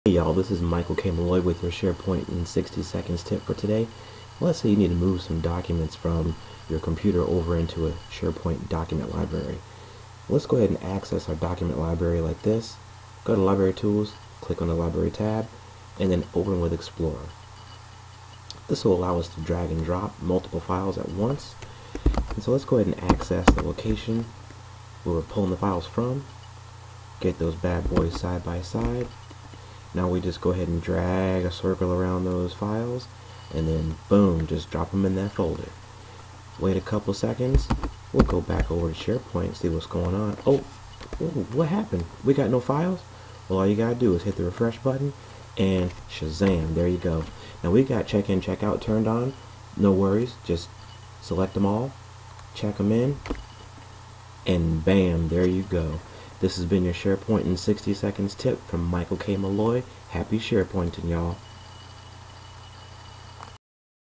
How To Upload Multiple Documents At Once To SharePoint Document Library
This video demonstrates how easy it is to drag and drop multiple files into a SharePoint document library. We used SharePoint 2010 for this demo.